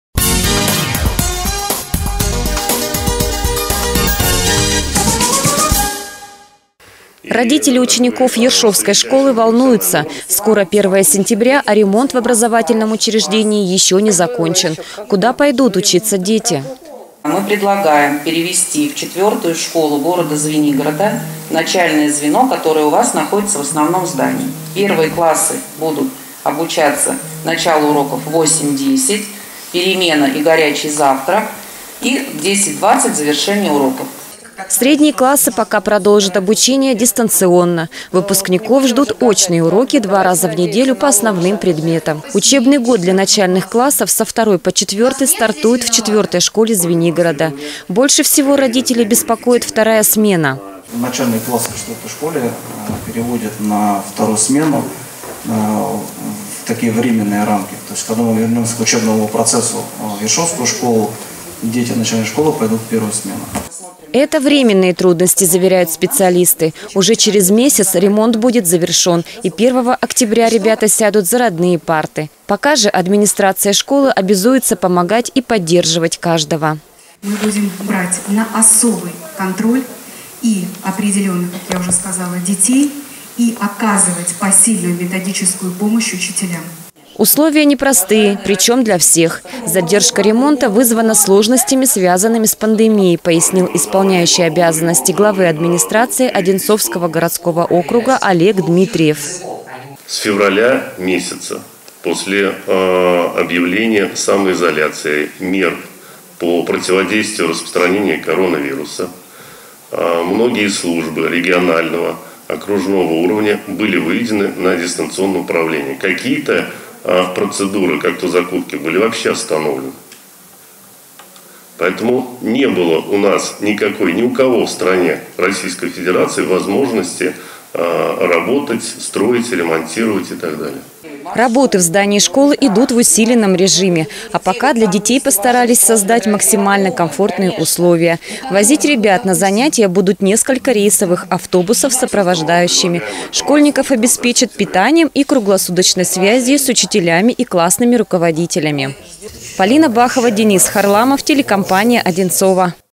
Непростое начало
Как и когда начнется учебный год в Ершовской средней школе? Ремонт здания не успели закончить, оно не сможет принять учеников 1 сентября.